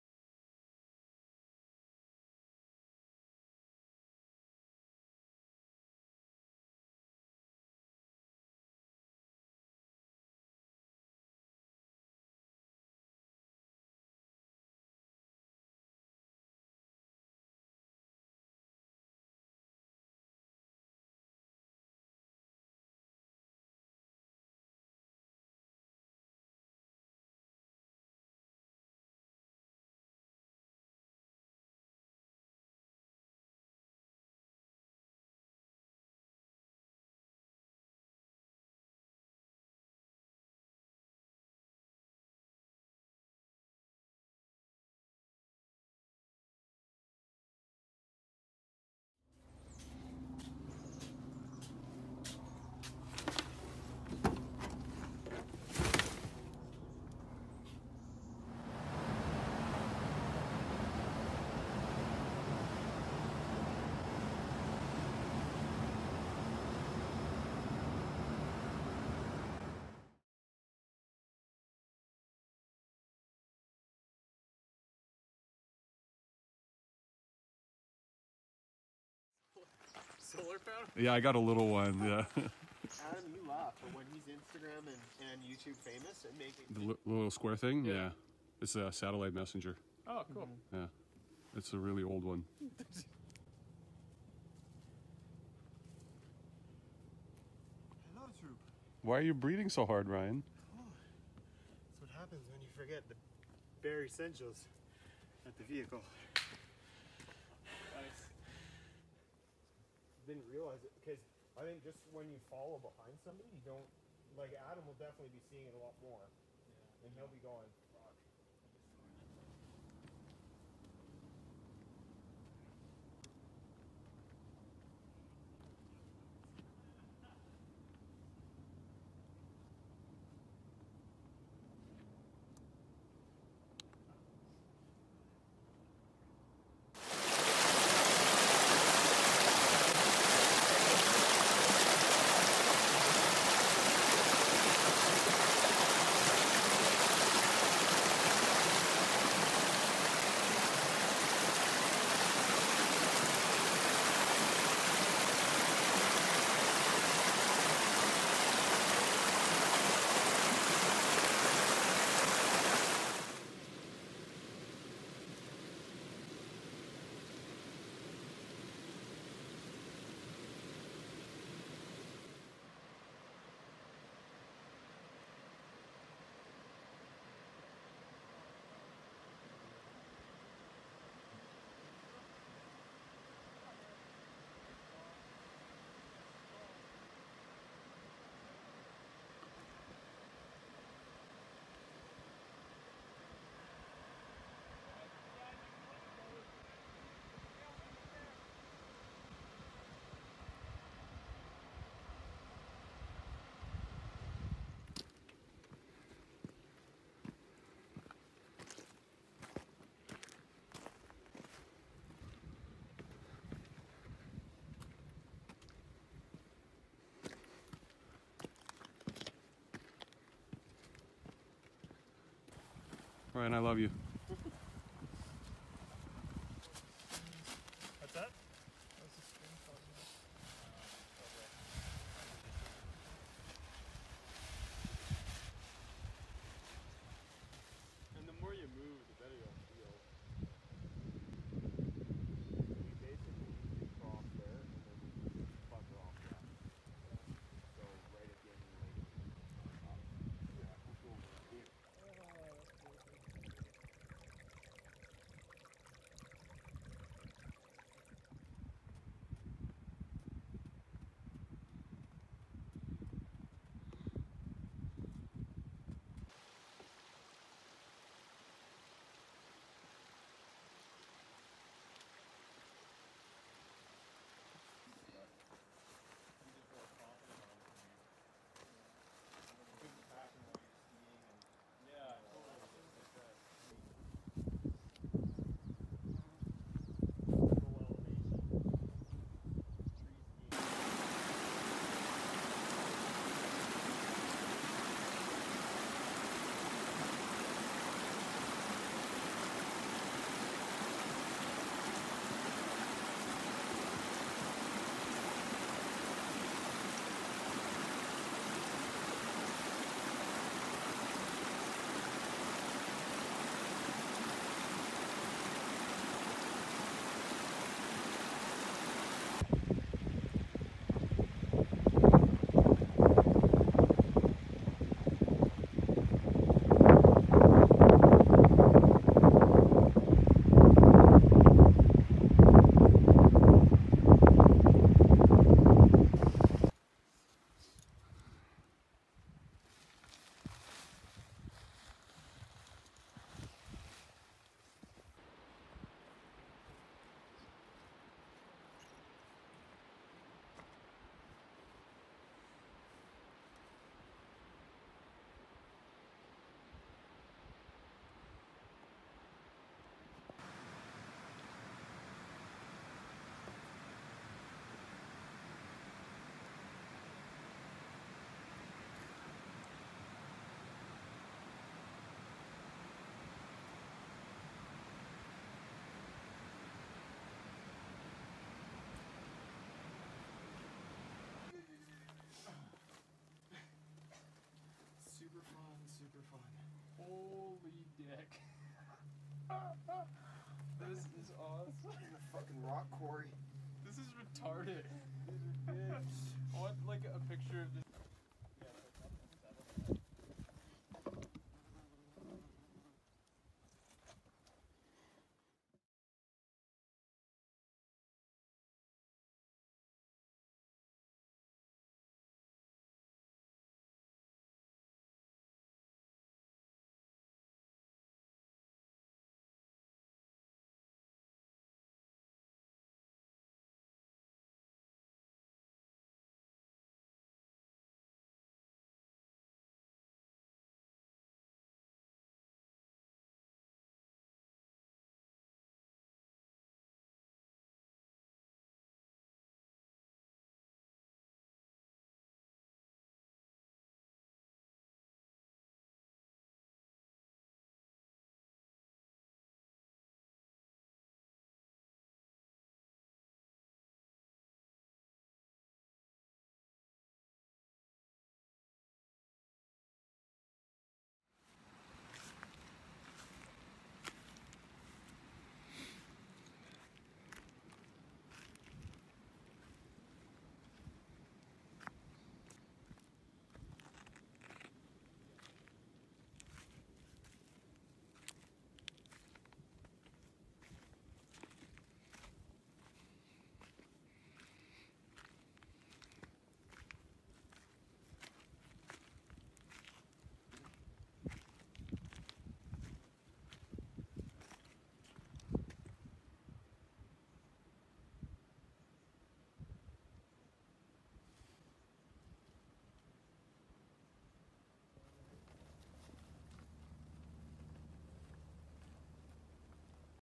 Hiking Mt Begbie, glacier and Tilley Lake.
We initially wanted to do Mark Berger Traverse, but it wasn't available due to forest fires. Our plan B (Mt. Fosthall) didn't pan out either so we decided to Mt Begbie near Revelstoke. It was one of the hardest hikes I've done, but camping in the alpine near Tilley lake was amazing.

I had ambitious plans for this video, with voiceovers and whatnot, but clearly it didn't pan out. It's been nearly a year. I'm gonna publish it the way it is. Hope you enjoy.